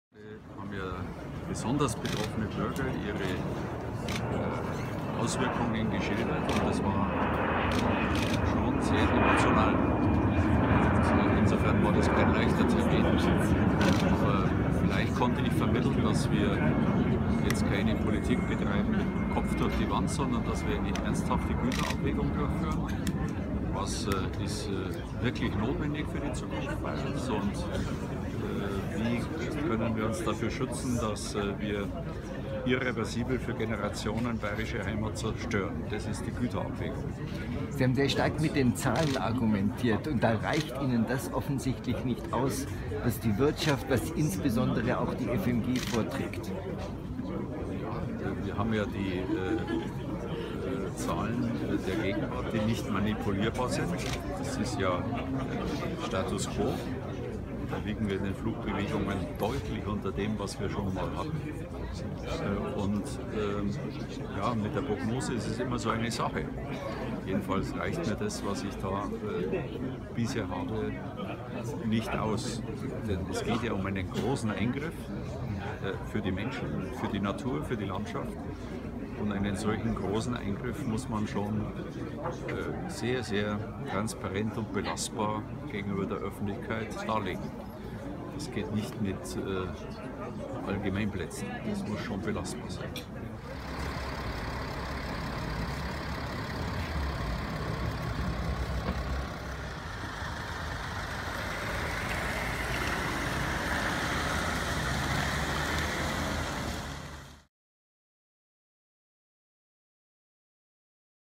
Seehofer-Besuch in Attaching: Das Fazit des Ministerpräsidenten
Rund 90 Minuten hat sich Ministerpräsident Horst Seehofer am Donnerstag, 29. Oktober, im Attachinger Sportheim die Argumente von 17 schwertsbetroffenen Anwohnern angehört, dann trat er vor die Presse und formulierte sein Fazit nach den Startbahn-Gesprächen. (Video: Armin Forster / Freisinger Tagblatt)